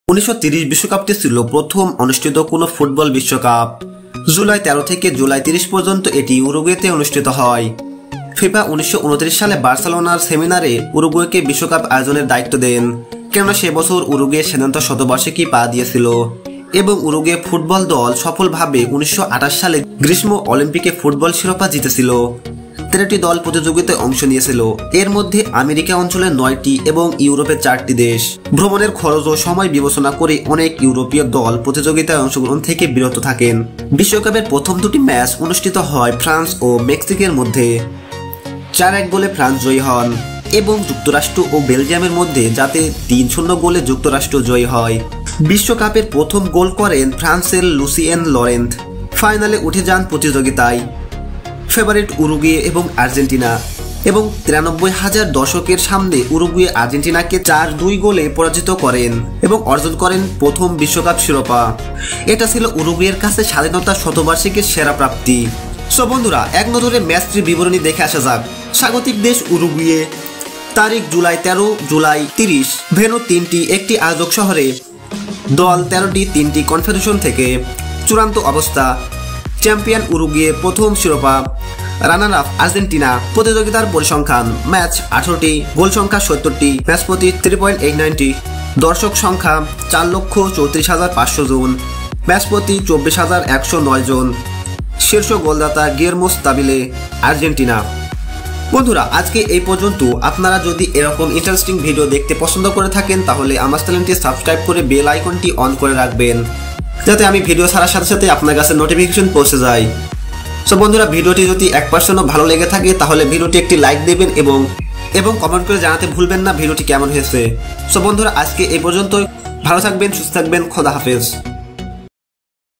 কেমন ছিল?? প্রথম fifa ওয়ার্ল্ড কাপ 1930.Hello world
১৯৩০ সালের ফুটবল বিশ্বকাপ ফাইনাল। Argentina VS Uruguay সম্পর্কে অজানা কিছু?